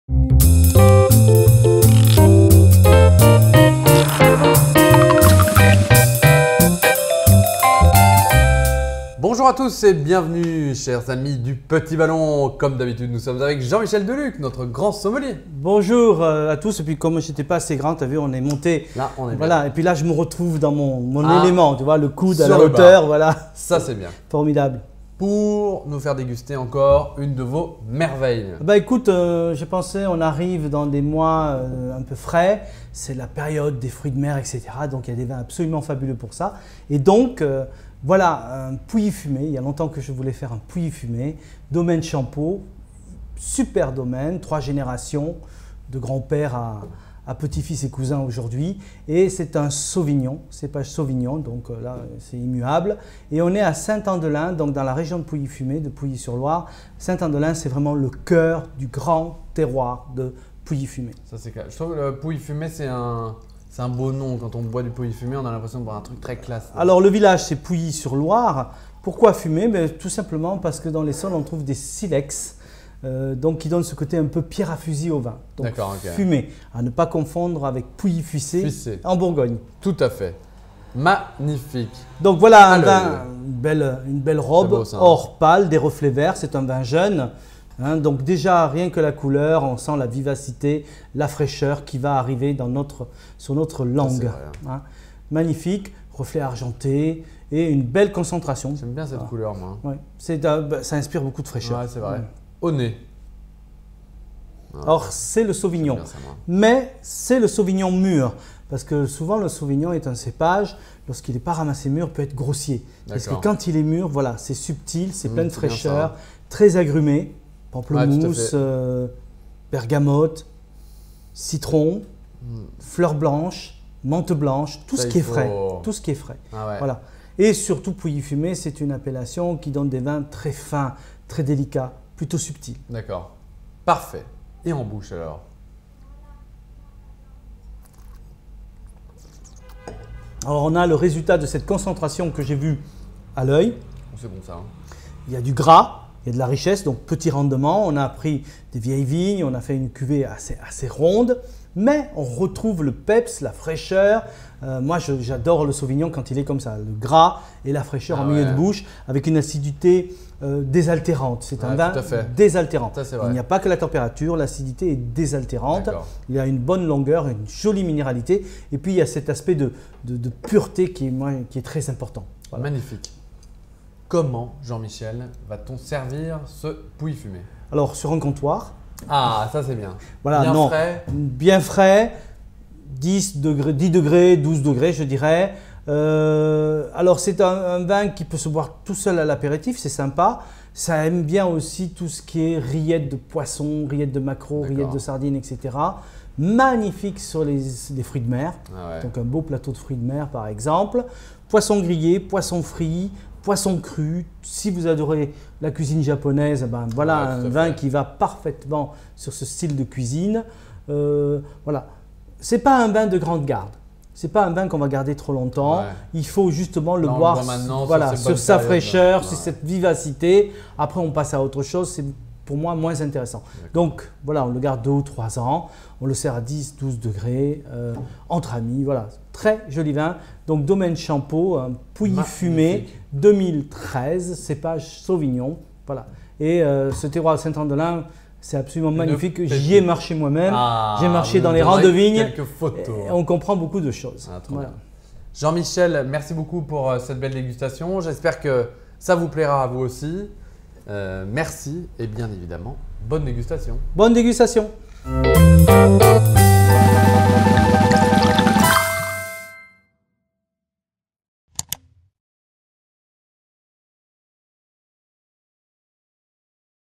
Pouilly Fumé 2013 | Robe pâle, aérien, désaltérant | Les dégustations du Petit Ballon
En octobre nous dégustons le vin blanc Pouilly Fumé 2013 du Domaine Champeau en compagnie de Martin et Jean-Michel Deluc, notre expert sommelier chez le Petit Ballon. Nos abonnés Âge de Raisin ont reçu ce vin en octobre.

L'abus d'alcool est dangereux pour la santé. Sachez consommer avec modération.